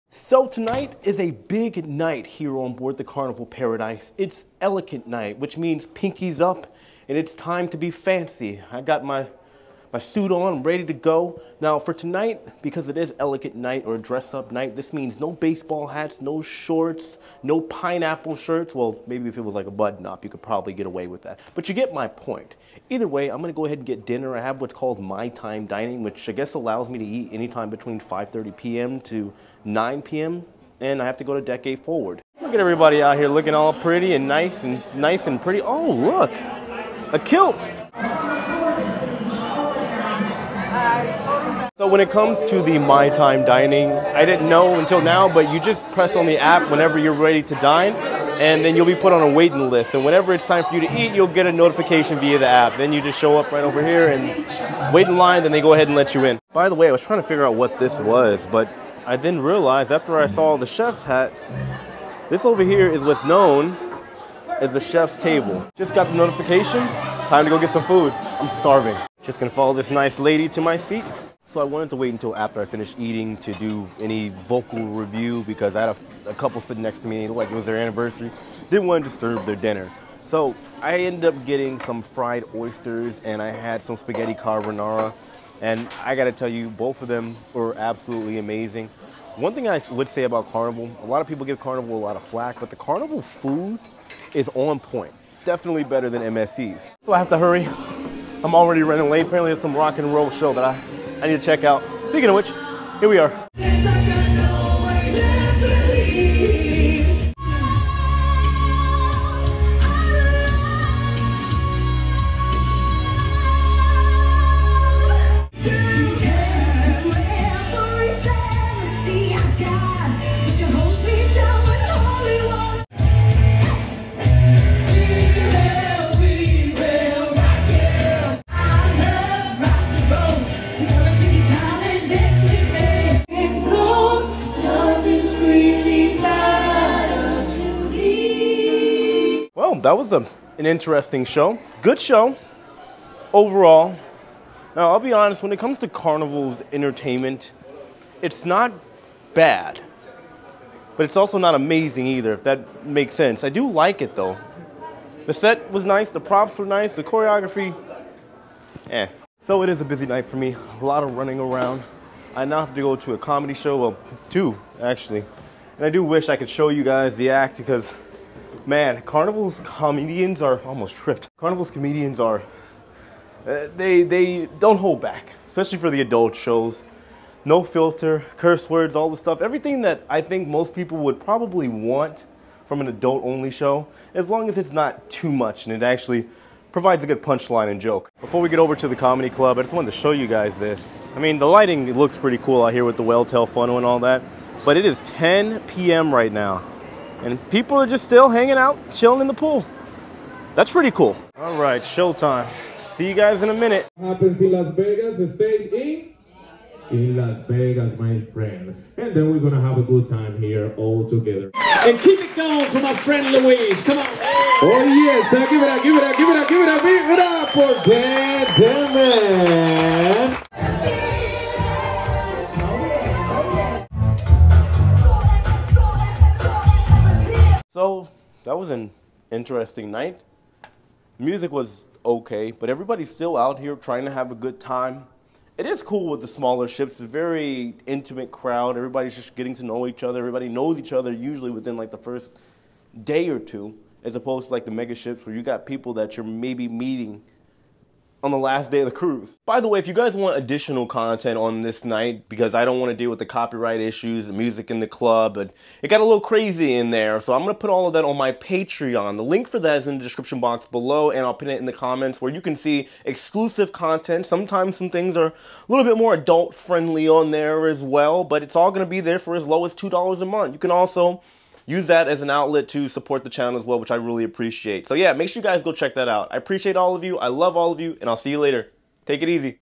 This Is What Fancy Night Looks Like On A Carnival Cruise Ship
Tonight is Fancy night on board the Carnival Paradise and you’re all invited!